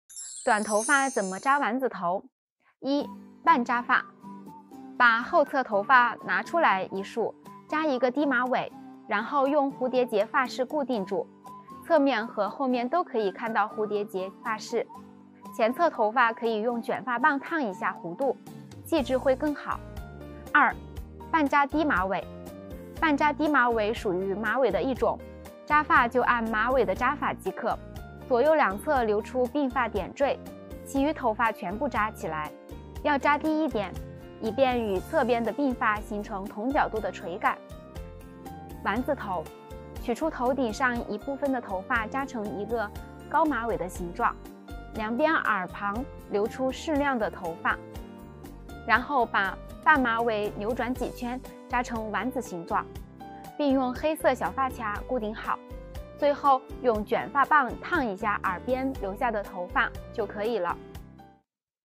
短頭發怎么扎丸子頭 短頭發扎丸子頭的方法
1、半扎發，把后側頭發拿出來一束，扎一個低馬尾，然后用蝴蝶結發飾固定住，側面和后面都可以看到蝴蝶結發飾。前側頭發可以用卷發棒燙一下弧度，氣質會更好。2、半扎低馬尾，半扎低馬尾屬于馬尾的一種，扎發就按馬尾的扎法即可，左右兩側留出鬢發點綴，其余頭發全部扎起來，要扎低一點，以便與側邊的鬢發形成同角度的垂感。